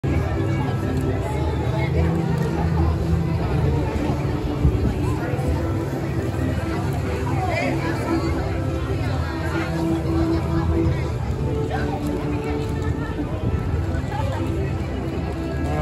Avengers Campus Captain America, new Black Panther, SpiderMan wave-DisneyCaliforniaAdventure 6/11/23
Avengers Campus Captain America, the new Black Panther, & Spider-Man wave from Avengers Headquarters balcony at night - Disney California Adventure 6/11/23